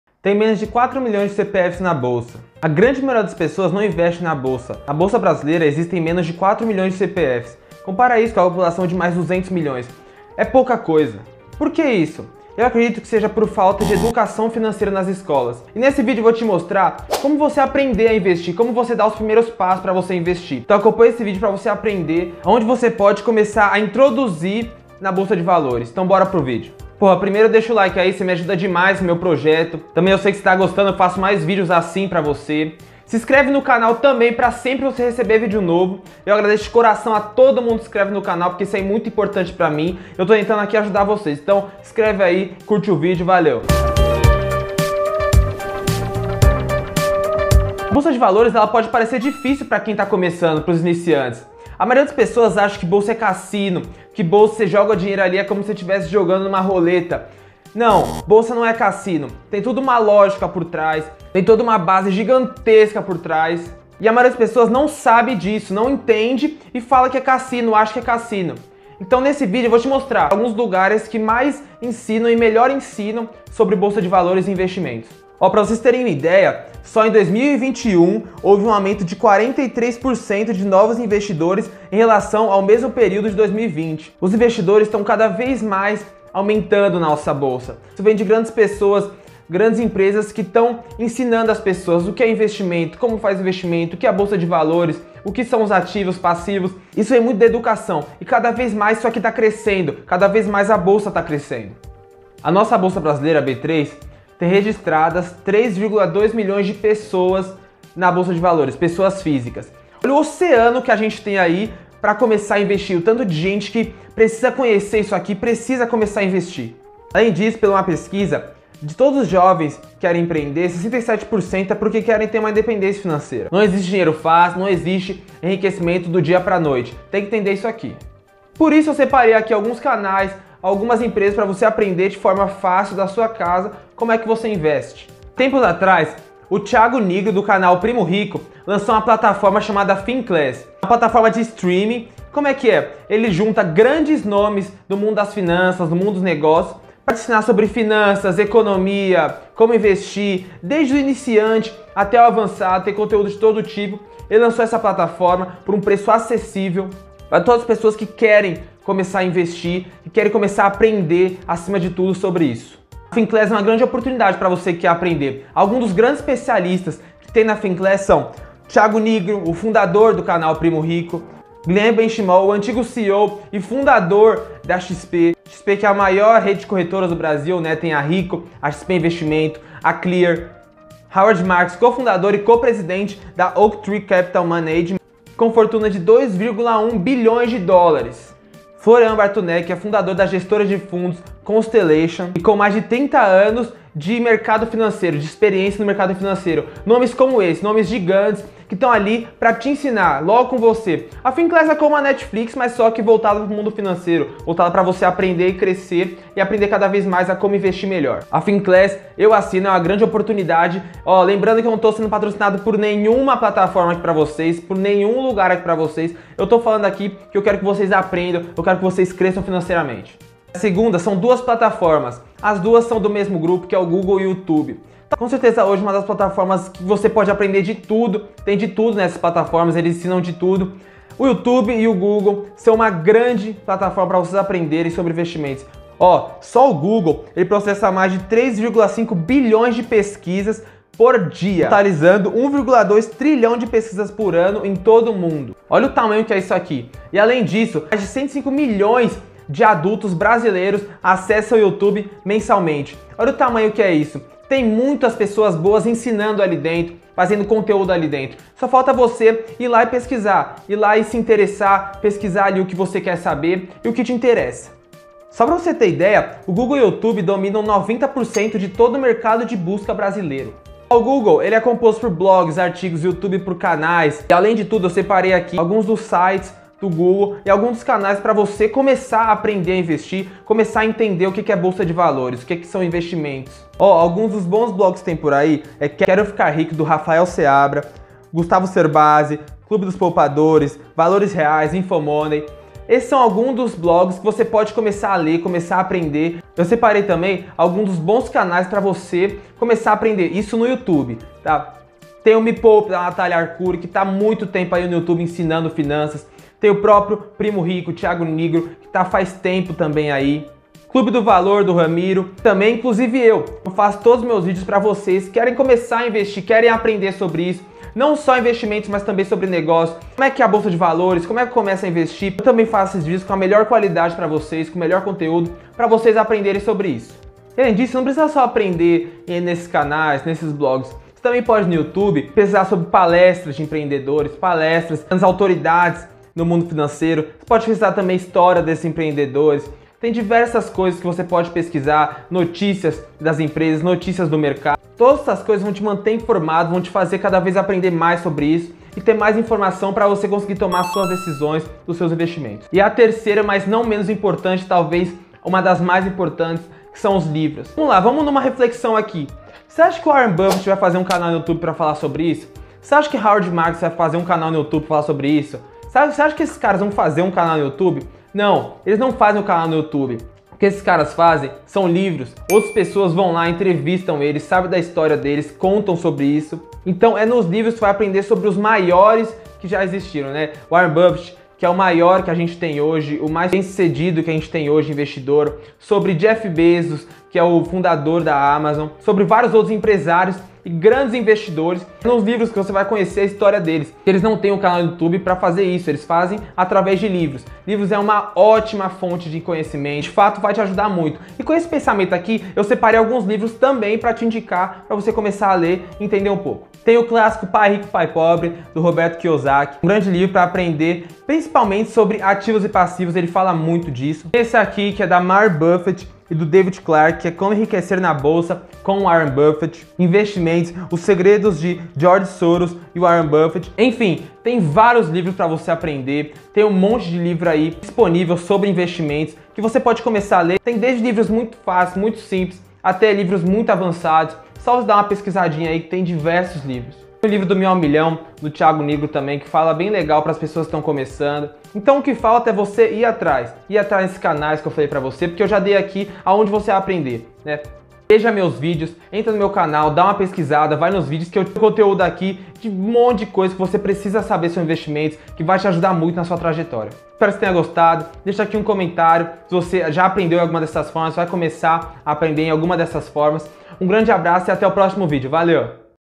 COMO APRENDER A INVESTIR NA BOLSA (Comece do jeito certo...)
Você deve estar ouvindo bastante falar de investimentos, desde ações até renda fixa, fundo imobiliário, mas provavelmente você não entende muito dessas coisas e não sabe onde aprender. Nesse vídeo eu te conto os melhores lugares para aprender sobre investimentos e começar sua jornada para sua independência financeira.
Mas não se engane, você tem que ver e estudar os ensinamentos, por que é isso que vai te dar a base pra começar a investir em ações.
Então acompanha esse vídeo e entenda onde aprender sobre investimentos!

00:00 - Como aprender a investir
01:18 - Dados do mercado brasileiro
02:16 - Finclass
03:44 - Google e Youtube
05:53 - Livros